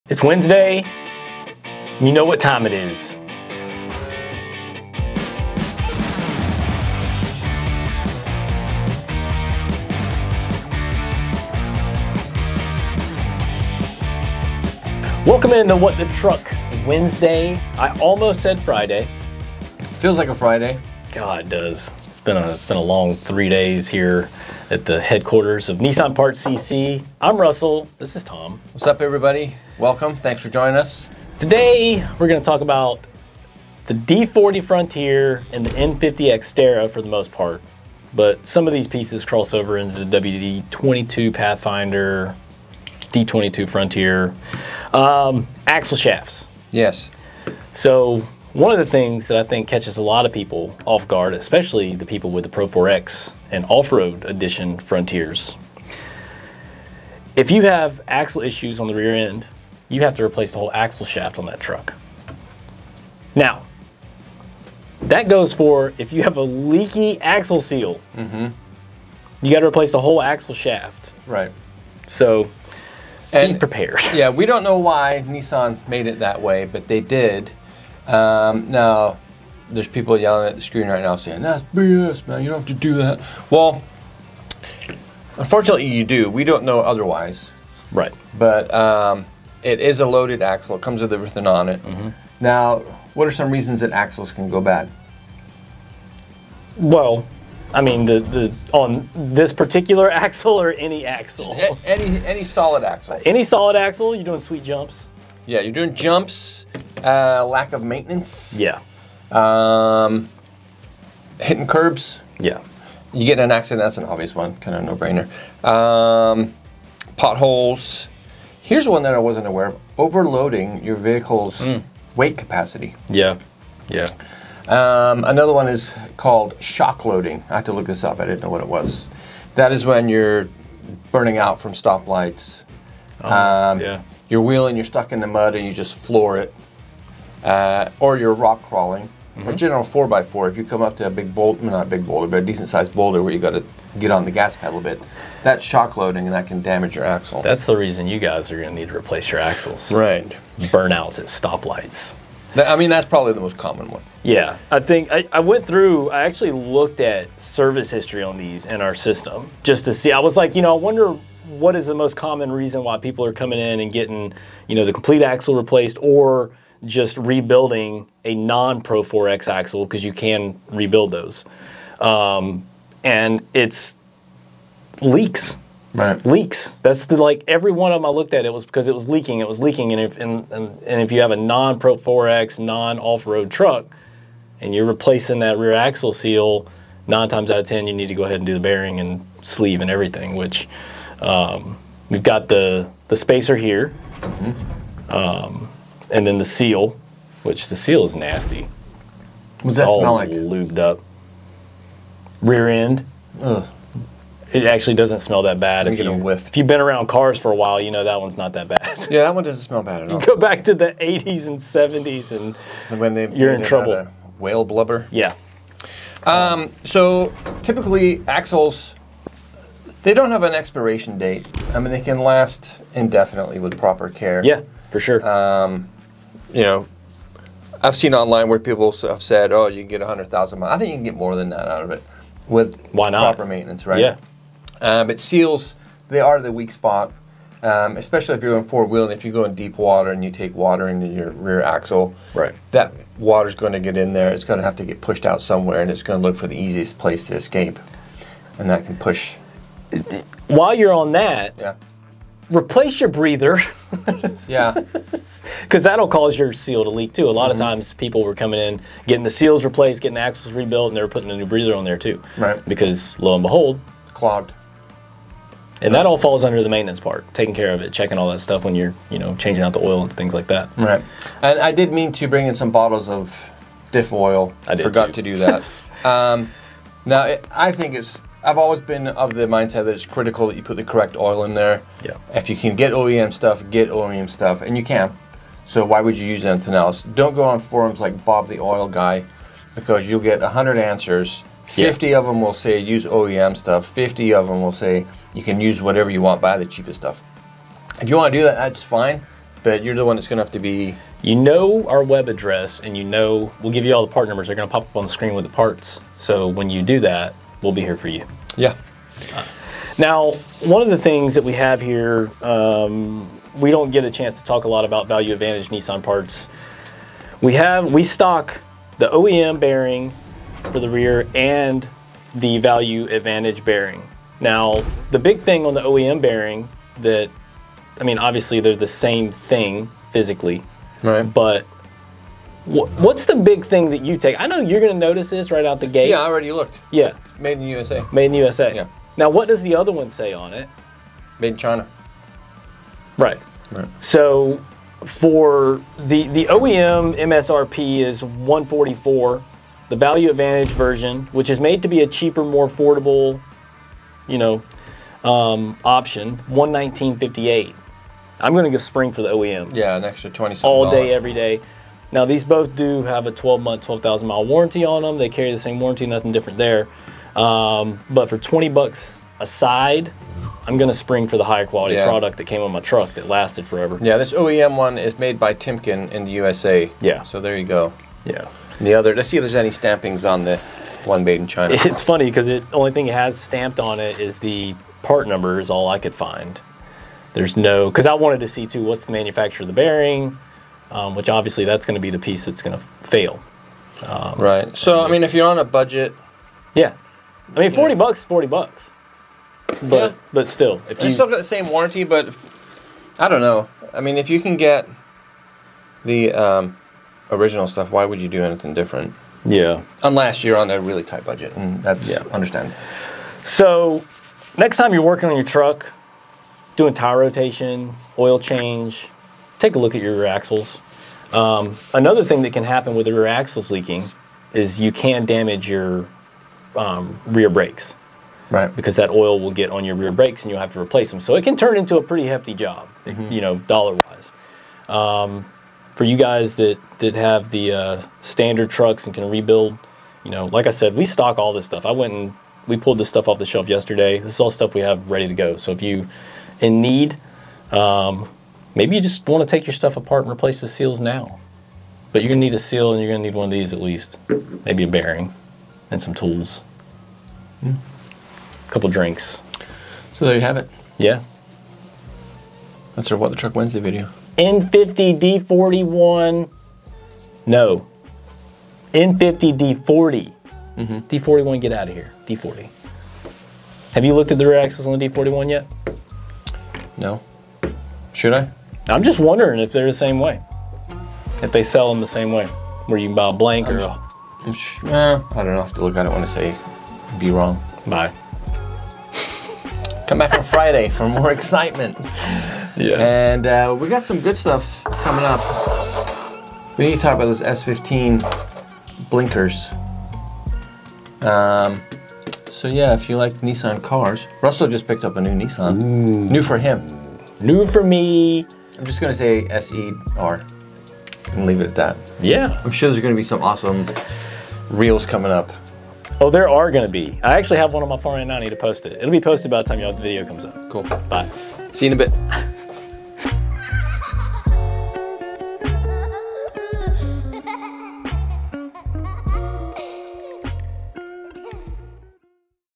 Nissan Xterra N50 and Frontier D40 Axle Shafts: The Complete Guide
In this video, we will go over the different rear axle shaft options available for the Xterra and Frontier, as well as the ways they are offered and common problems.

REAR AXLE SHAFT OPTIONS
The Xterra and Frontier are available with two different types of rear axle shafts:

-C200: This is the standard rear axle shaft for most Xterras and Frontiers. It is a lighter-duty axle shaft that is designed for on-road use.
-M226: This is a heavier-duty rear axle shaft that is designed for off-road use. It is stronger and more durable than the C200 axle shaft.

WAYS THEY ARE OFFERED
Rear axle shafts are offered in two ways:
S, SV, SL : Shaft, Bearing, Spacer, Seal, etc
Pro-4X : Axle Shaft Assy

COMMON PROBLEMS WE SEE
-The most common problem with rear axle shafts is that they can bend or break if they are overloaded or subjected to excessive stress. This can happen when off-roading or when towing a heavy trailer.

-Wear and tear: Over time, rear axle shafts can wear and tear. This can cause them to become loose or noisy.
Seal failure: The seals on rear axle shafts can fail over time, allowing oil to leak out. This can lead to premature wear and tear on the axle shaft.

-Corrosion: Rear axle shafts can corrode over time, especially if they are exposed to salt water or harsh chemicals. This can weaken the axle shaft and make it more likely to fail.